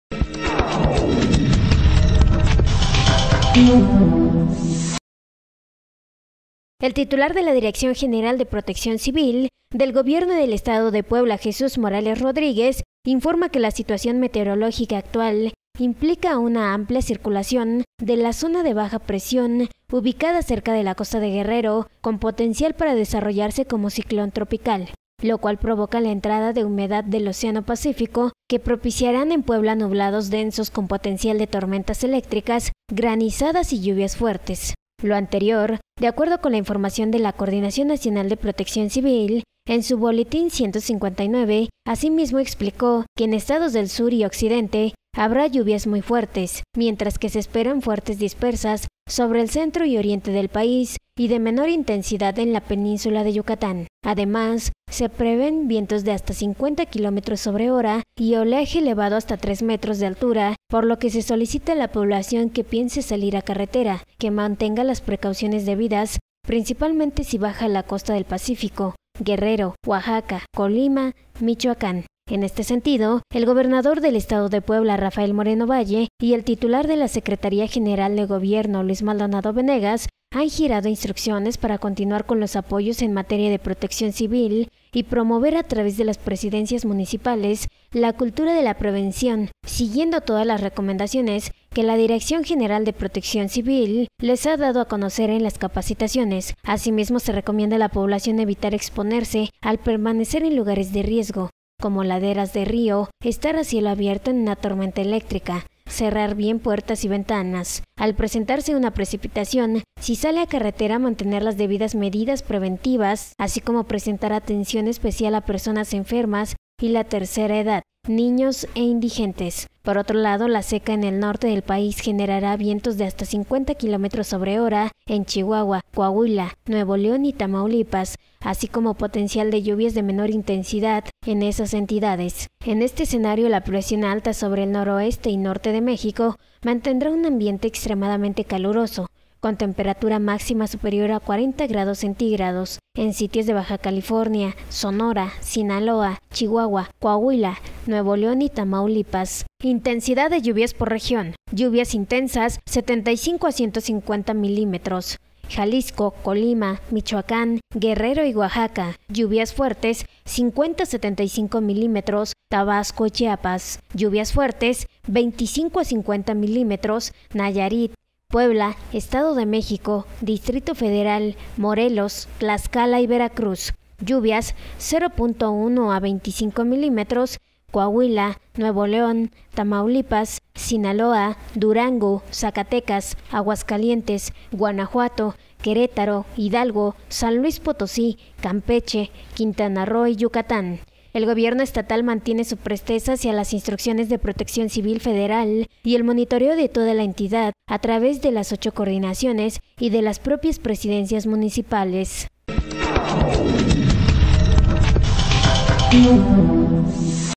CONDICIONES METEOROLÓGICAS
El titular de la Dirección General de Protección Civil (DGPC) del gobierno del Estado de Puebla, Jesús Morales Rodríguez informa que  la situación meteorológica actual implica una "amplia circulación de la zona de baja presión ubicada cerca de la costa de Guerrero con potencial para desarrollarse como ciclón tropical, lo cual provoca la entrada de humedad del océano Pacífico, que propiciarán en Puebla nublados densos con potencial de tormentas eléctricas, granizadas y lluvias fuertes".